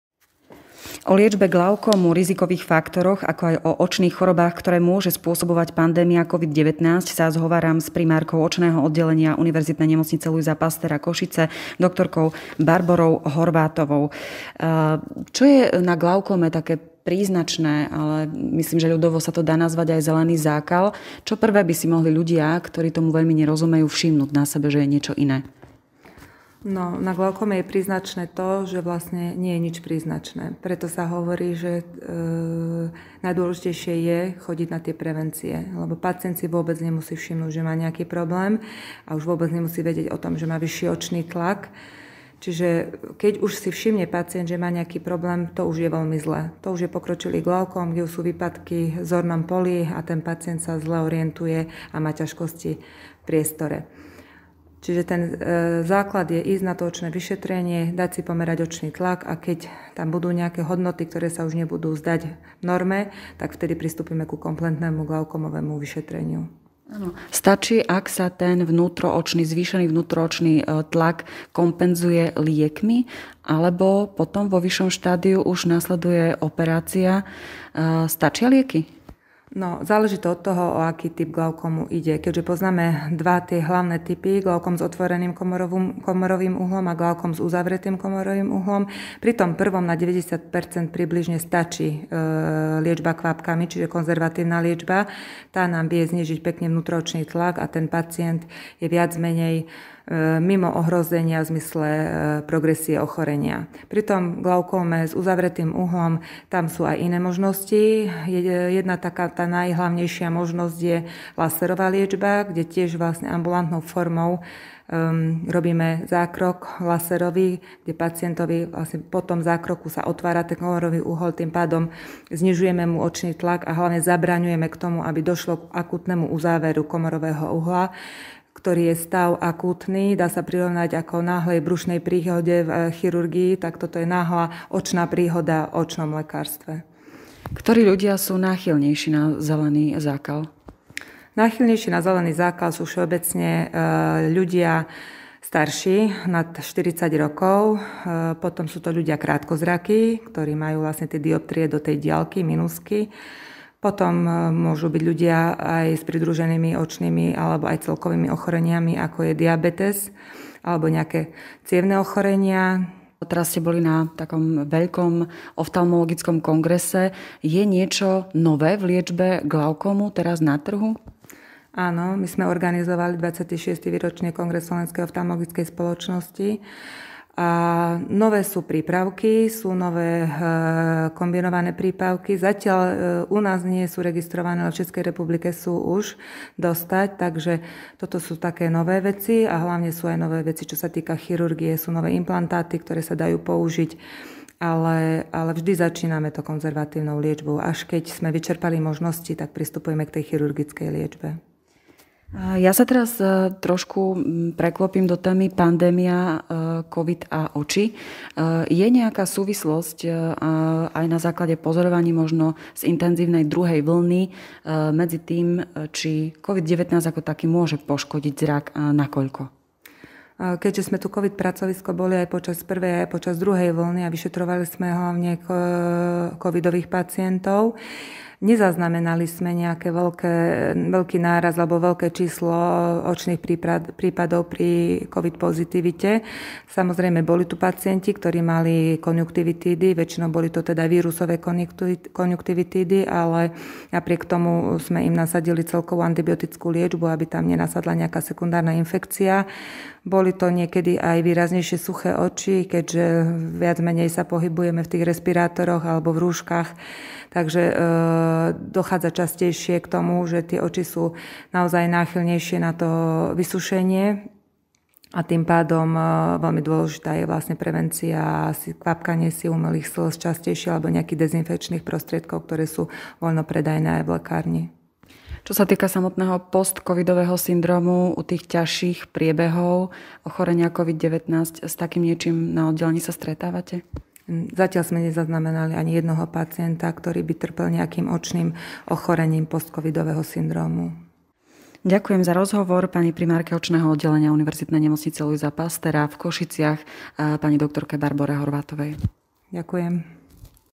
O glaukóme s primárkou Očného odd. MUDr. B. Horváthovou | PODCAST
ZELENÝ ZÁKAL - GLAUKÓM
👉 keď spozorujete príznaky, už je neskoro
👉 najdôležitejšia je prevencia - pravidelné očné vyšetrenia
👉 nové medicínske prípravky
👉 moderné chirurgické implantáty
Možnosti liečby sú široké. Vypočujte si PODCAST s primárkou Očného oddelenia UNLP na Rastislavovej ulici MUDr. Barborou Horváthovou.